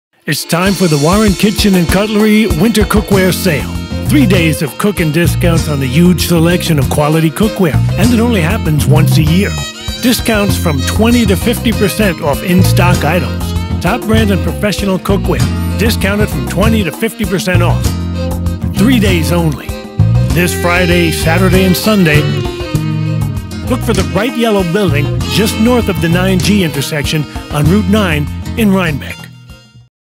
Warren Kitchen Cutlery Winter Cookware Sale 2018 ProRes
Rough cut of Winter Cookware Sale 30 scond TV spot (hard cut transitions) ProRess 422 version for cable